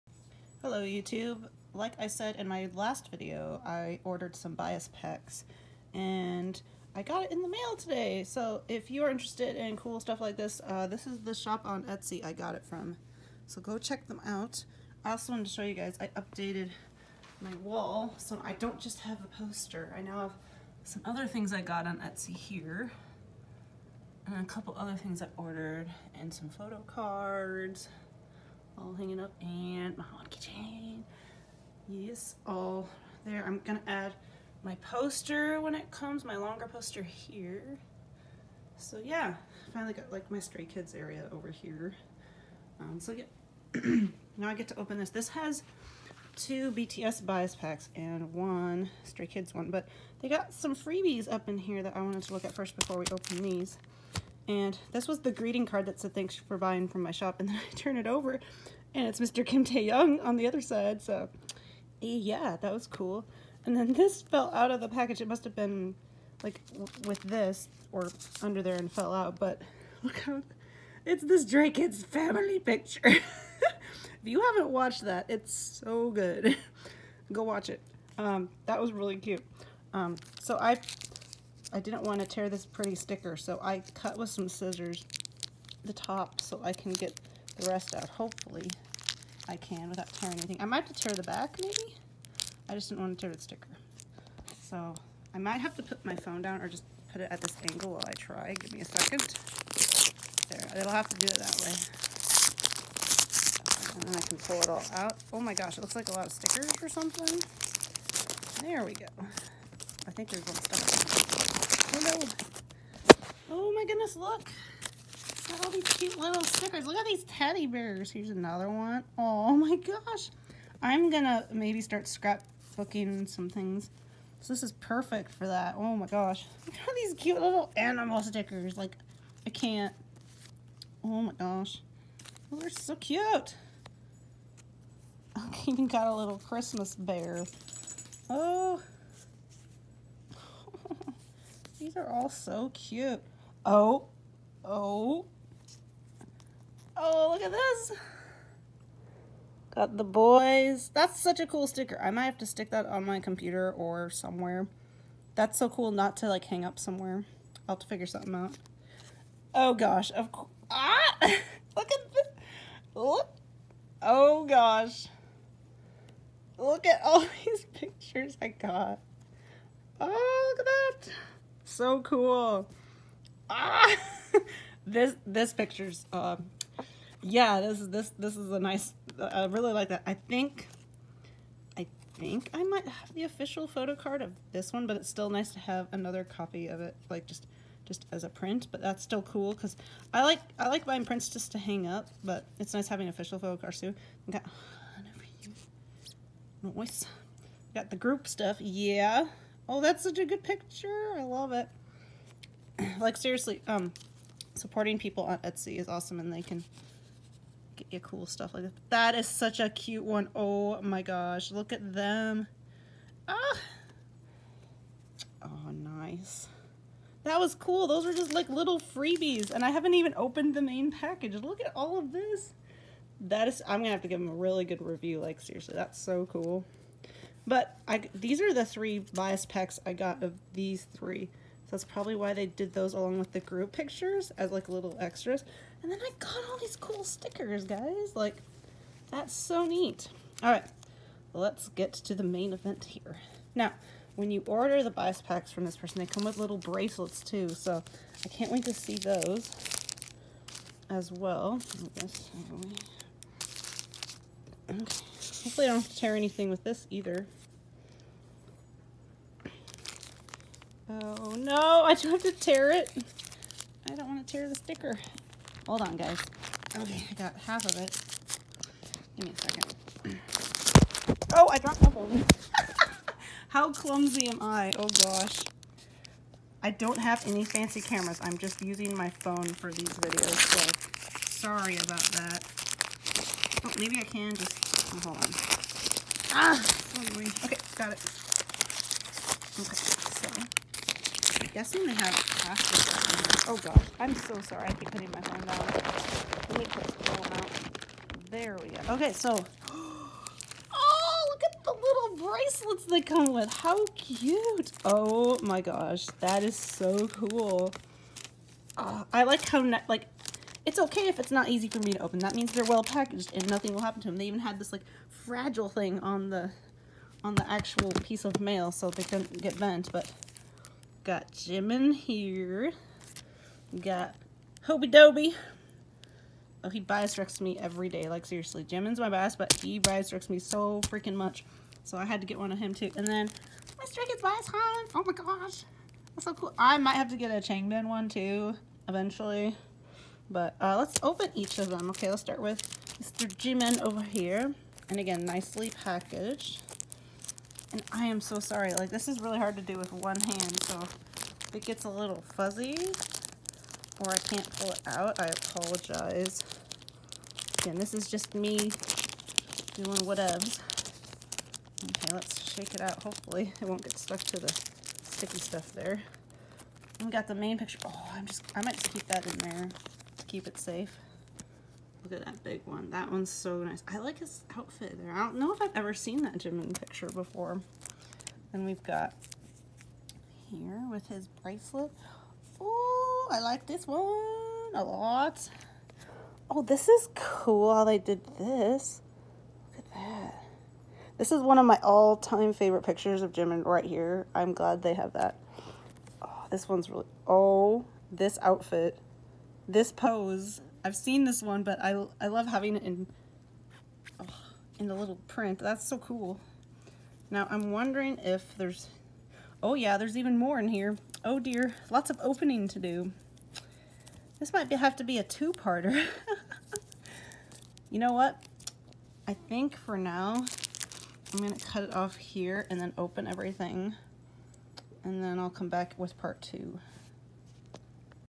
Kpop bias packs: part 1!
I received some awesome BTS and SKZ bias packs in the mail via an awesome Etsy seller!